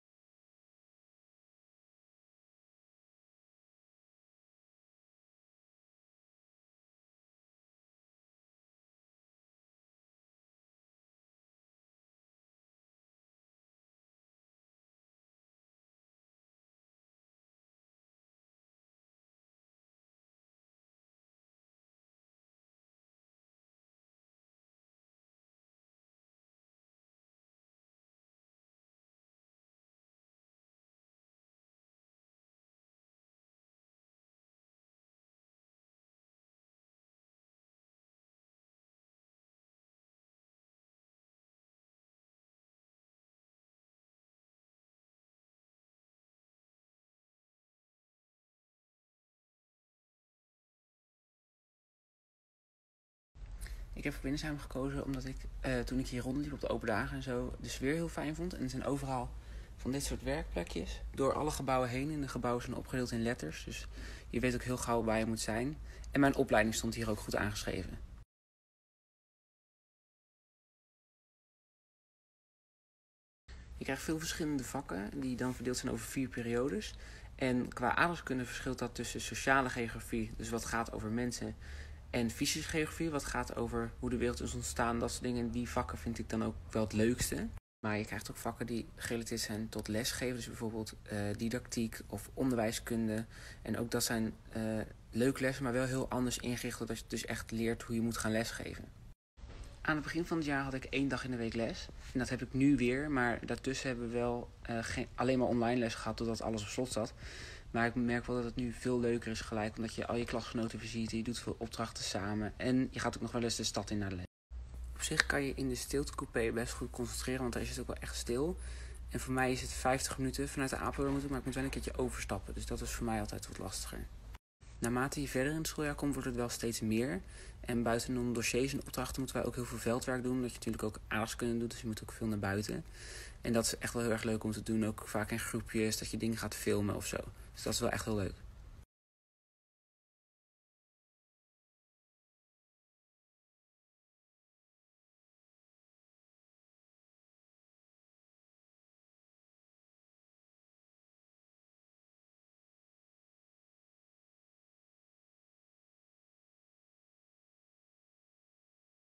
LO Aardrijkskunde - Ruben Kuus | Instagram Take-Over
Bekijk een dag uit het leven van een student LO Aardrijkskunde. Ruben laat zien wat hij zoal doet en beantwoordt vragen van volgers via Instagram. Volg je ons nog niet op Instagram? Check: